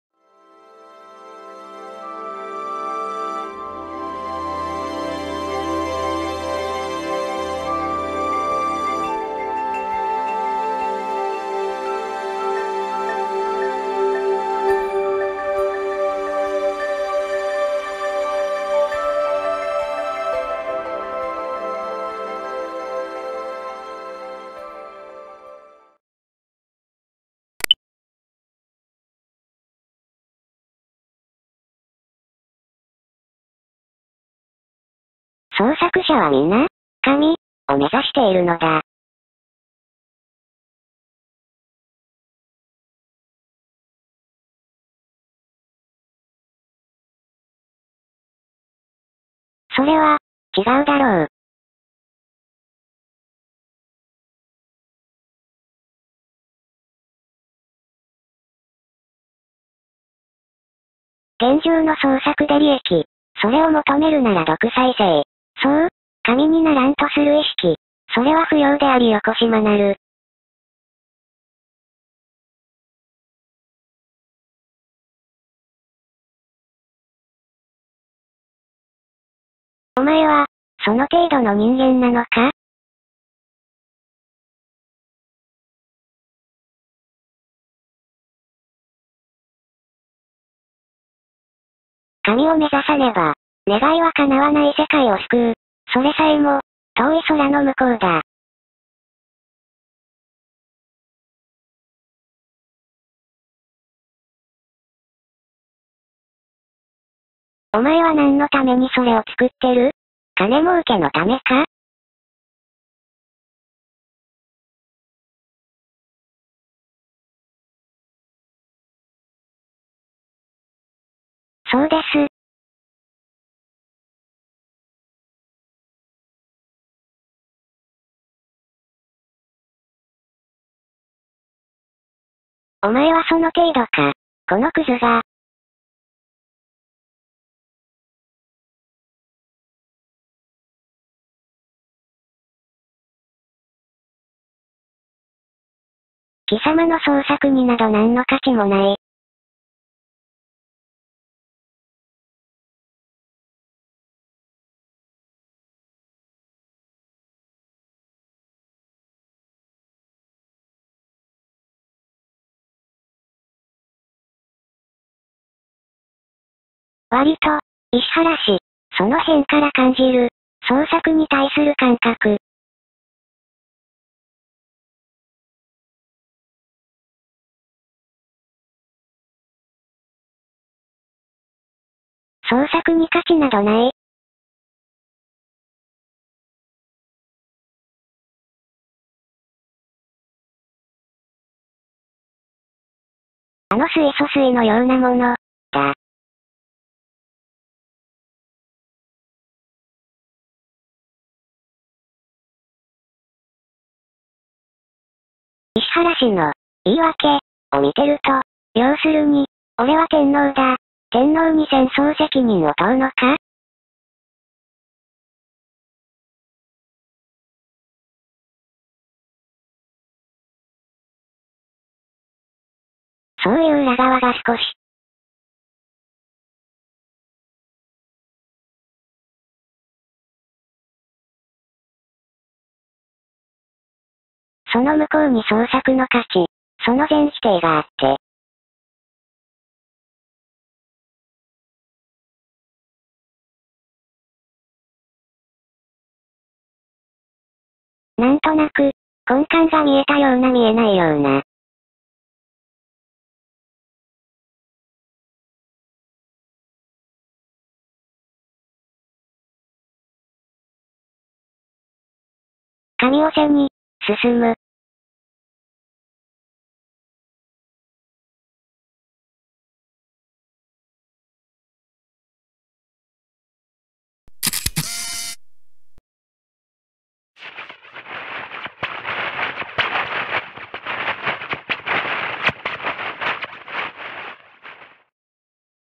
ショートサーキット出張版読み上げ動画2294
天皇陛下と首相の違いとは？東京都知事の不正に対して首相の、
監督責任はどの程度か。太平洋戦争、天皇の責任とは果たして。

素材著作
風景